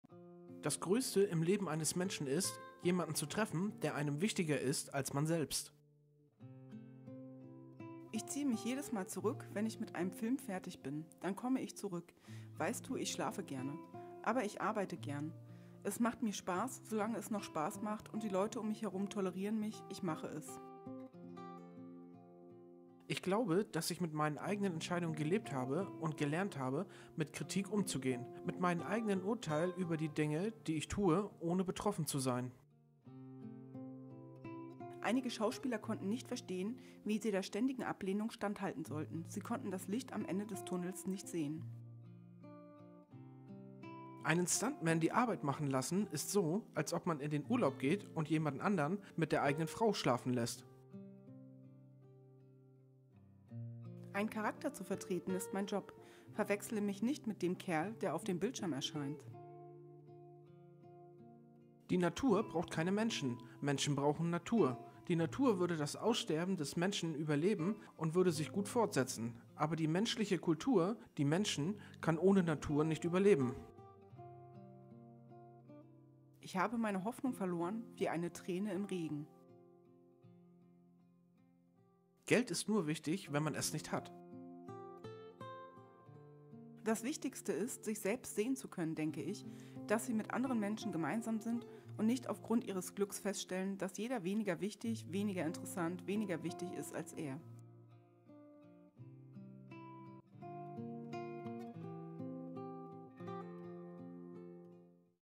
HARRISON FORD über Ablehnung und Motivation | Persönlichkeitsentwicklung | Zitate zum Nachdenken
Harrison Ford ist ein US-amerikanischer Schauspieler und Produzent.
Er war unter anderem in fünf Filmen der Reihe "Star Wars" als "Han Solo" zu sehen und ist einer der kommerziell erfolgreichsten Darsteller der Filmgeschichte.
Ebenfalls populär wurde er in der Rolle des Archäologen und Abenteurers "Indiana Jones", den er seit 1981 in vier Filmen darstellte.
Für seine Rolle in "Der einzige Zeuge" erhielt er 1986 eine Oscar-Nominierung.

Wir wünschen dir viel Spaß und Freude mit Harrison Ford´s Zitaten ❤